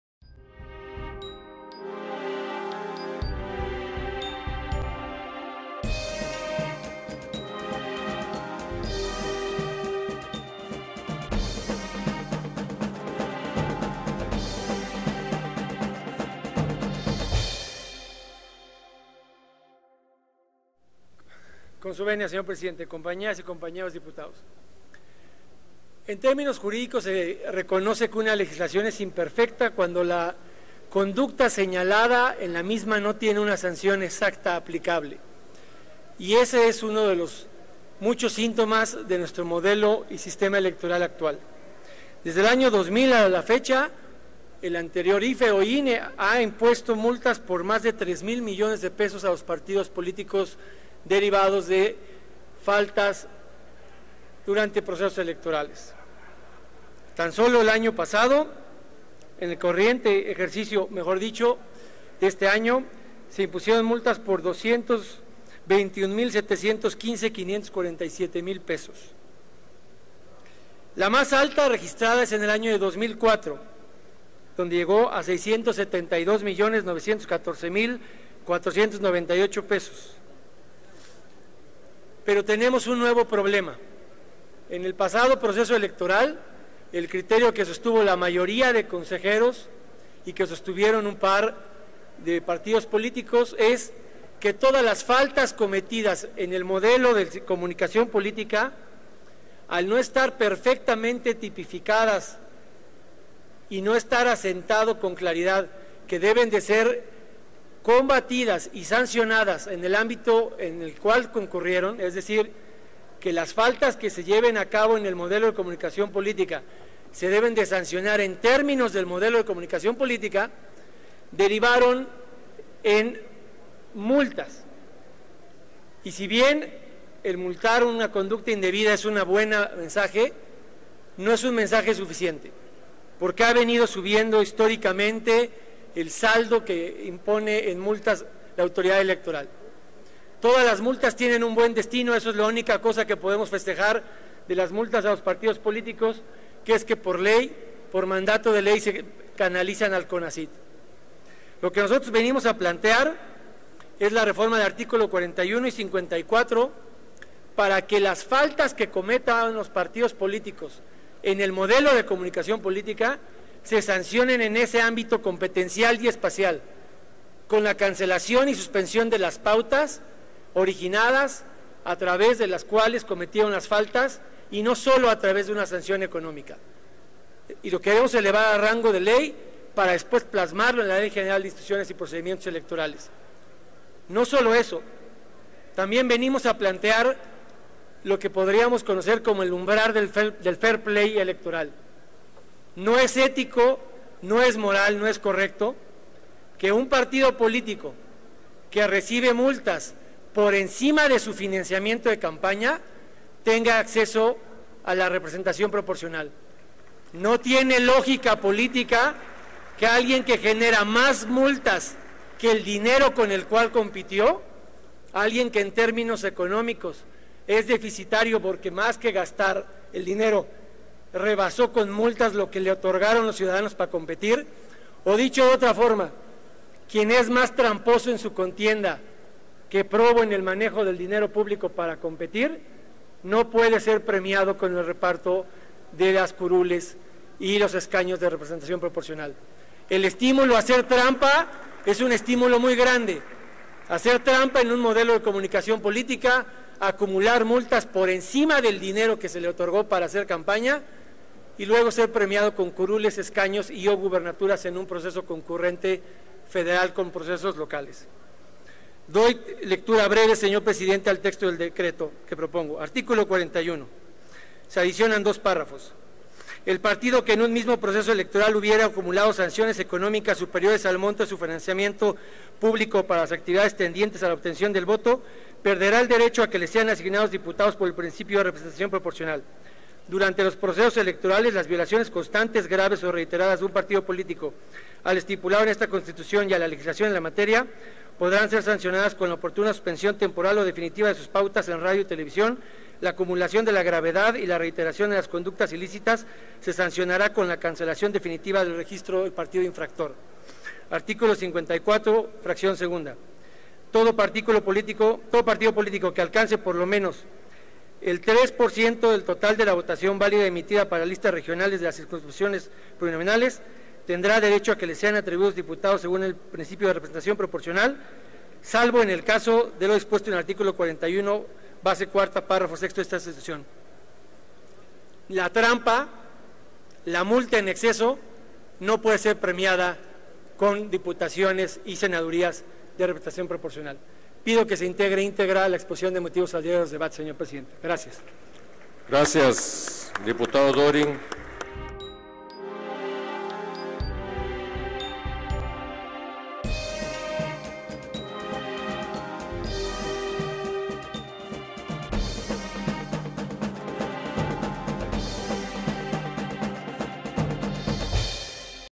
Dip. Federico Döring (PAN) - Iniciativa
La iniciativa del Dip. Federico Döring Casar (PAN) tiene por objeto establecer penas a los partidos políticos que acumulen sanciones económicas o realicen violaciones constantes y reiteradas durante un proceso electoral.